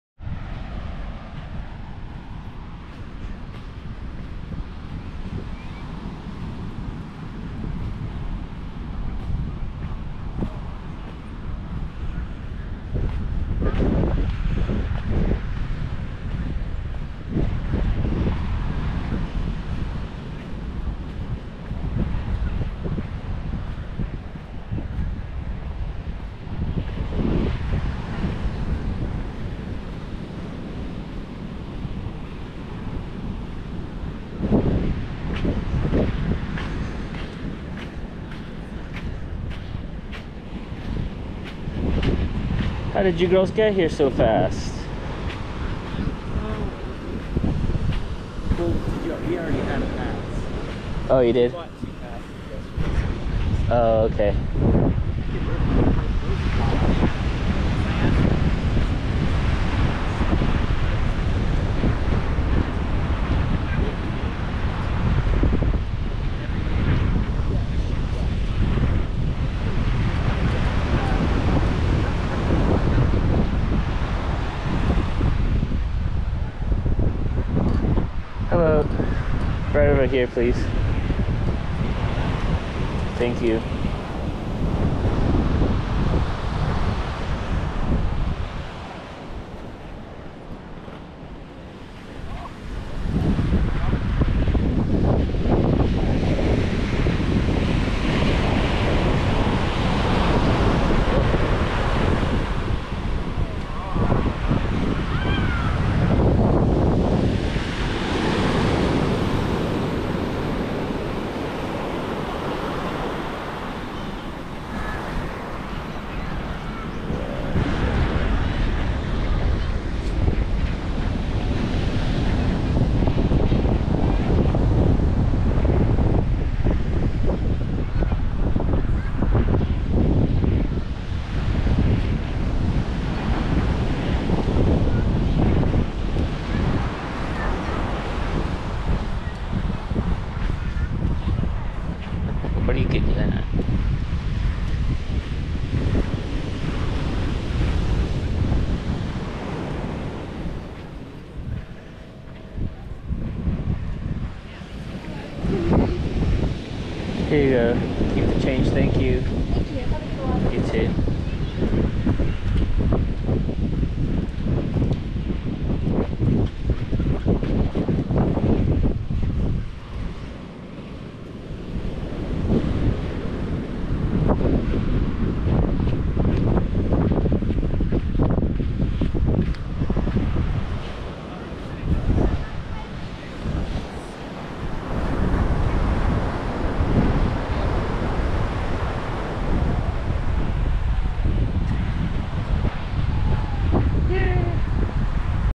Arriving at Poodle Beach in Rehoboth, Delaware
Arriving at Poodle Beach in Rehoboth, Delaware on Tuesday, June 18, 2024.